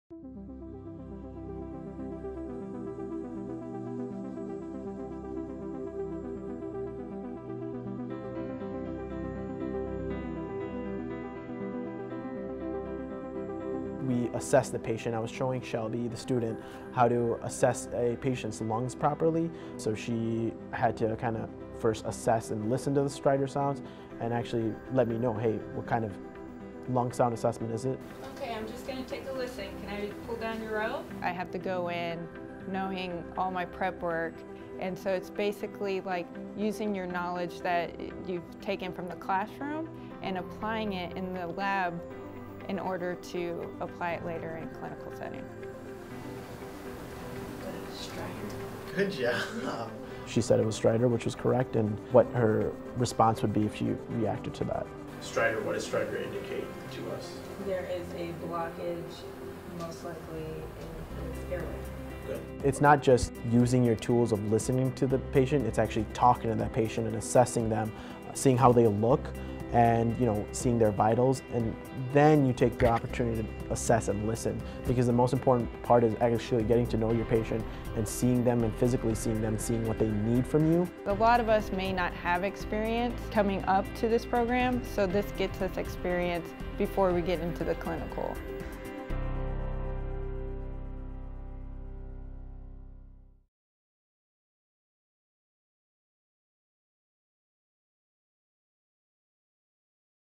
Inside a Real Nursing Simulation Lab Demonstration
During a nursing simulation lab demonstration, Direct Entry MSN students take the skills they have learned online and in skills lab and apply them to how they would work in a clinical situation.

“Your clinical instructor gives you a sim lab scenario, then you go in and interact with a manikin but you have someone behind the scenes guiding the scene so it allows you to practice situations that you’re not prepared for,” says Shelby, a Direct Entry MSN student.

In this video, Shelby and her clinical professor, Tobin Tito, perform a nursing simulation lab demonstration. The sim lab scenario was a patient who has undiagnosed stridor, a high-pitched wheezing sound caused by disruptive airflow.

“We showed the student, Shelby, how to assess a patient’s lungs properly,” says Tobin Tito. “First had to assess and listen to the strider sounds and provide a lung assessment. She said it was stridor, which was correct, and what her response would be if you reacted to that.”
Nursing simulation lab demonstrations, like the one in this video, allow students to gain diverse opportunities in a safe and supportive environment that allow them to learn effectively. Sim lab scenarios can include everything from infants to older adults, to a veteran scenario, to dealing older people and dealing with families and more complex situations.

“My role is to educate students and prepare them for the real world,” says Tobin. “From how to actually treat a patient, to how to do certain skills like starting an IV giving medications - that’s what we learn in nursing simulation labs. It’s the fundamentals about how we treat a patient, assess a patient, and do some nursing interventions, according to what you see with patients.”

To learn more about nursing simulation lab demonstrations and what sim lab scenarios you can experience as a student in Marquette’s Direct Entry MSN program,